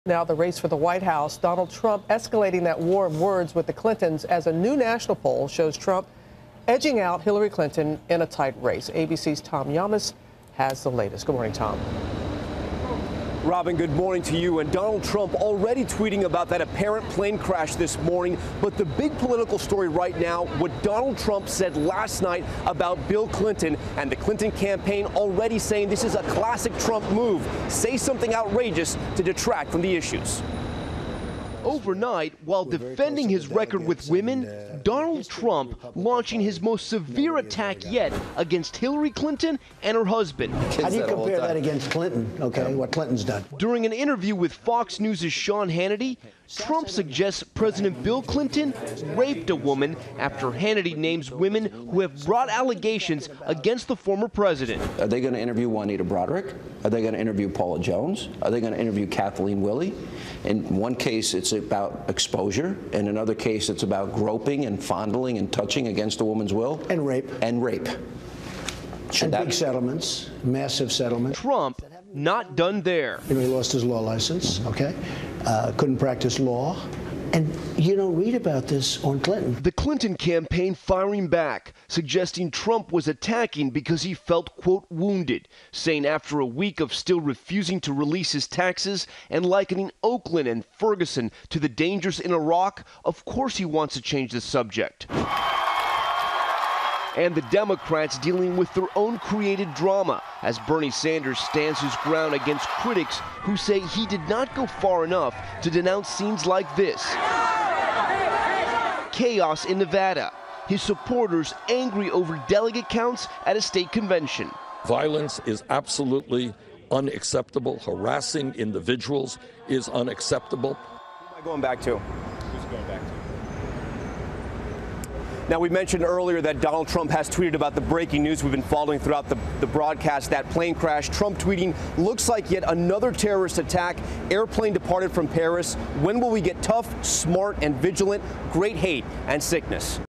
Donald Trump Launches Harshest Attacks Yet on Bill Clinton
Trump suggested the former president allegedly lost his law license and raised allegations of Clinton and women.